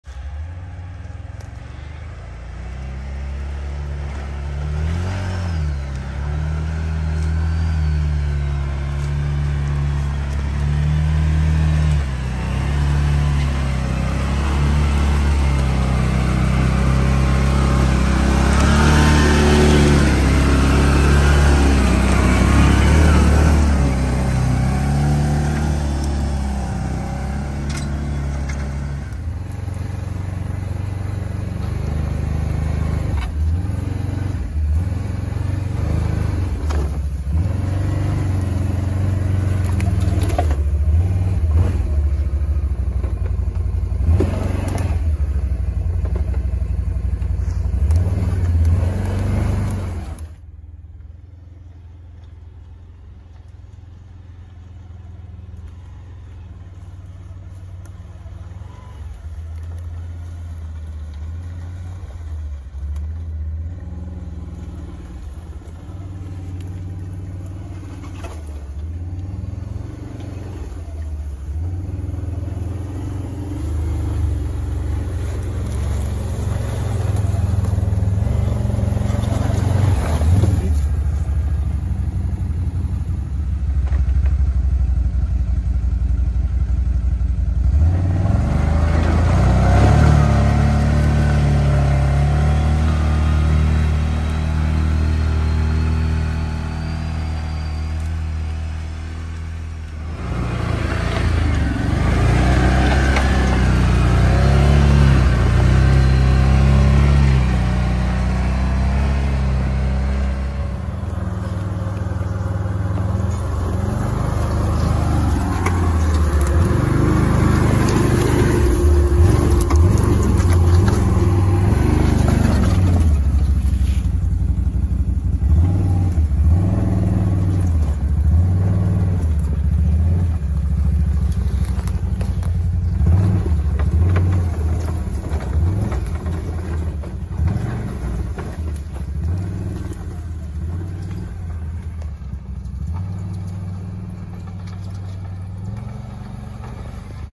Canam Outlander 6x6 - Springtime with tracks
Brief and slow rides on steep, somewhat rough and uneven terrain. During springtime.